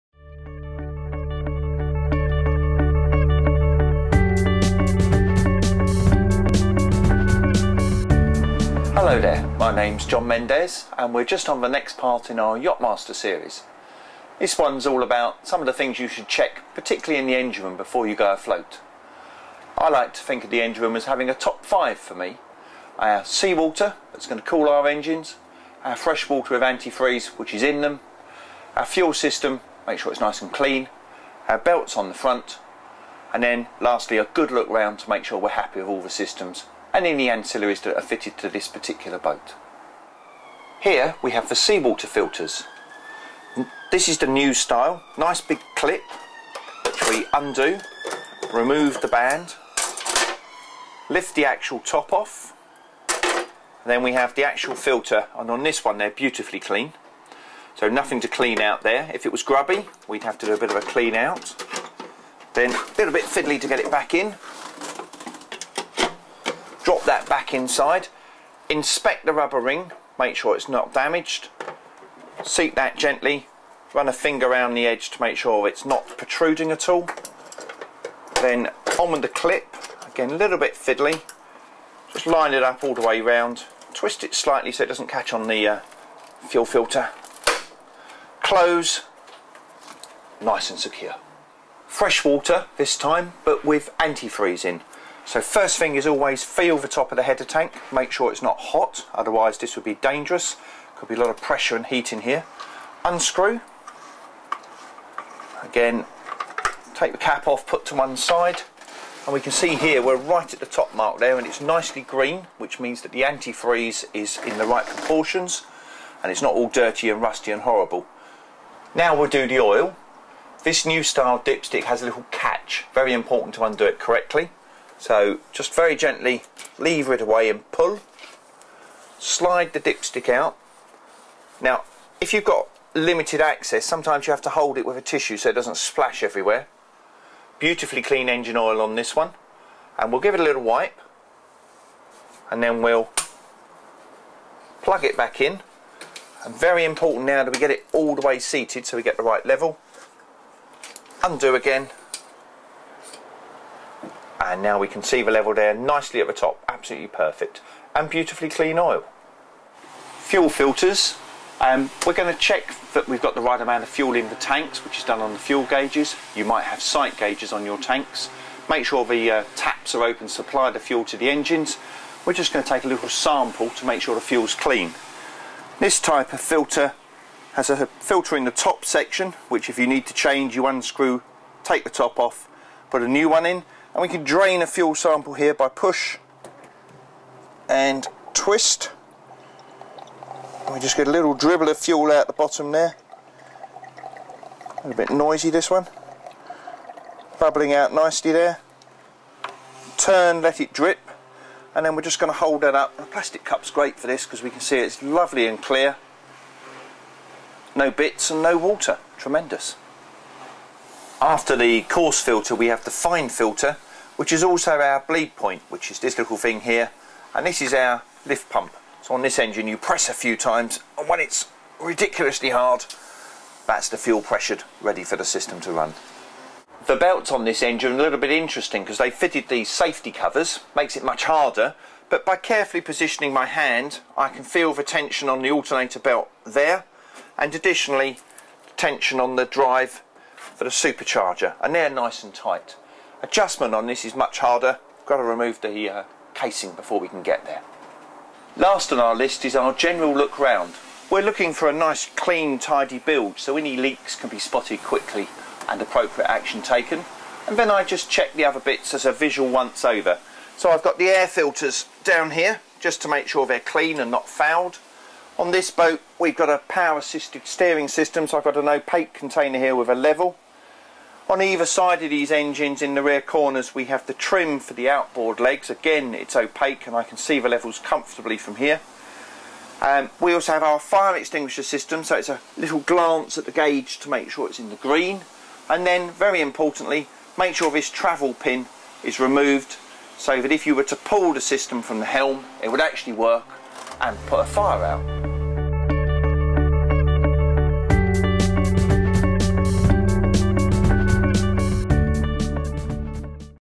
Motor Boat & Yachting's Yachtmaster training - engine checks
Follow Motor Boat & Yachting as we show you how to become a Yachtmaster. See the 16th part of the series about engine checks in the November 2011 issue of the magazine.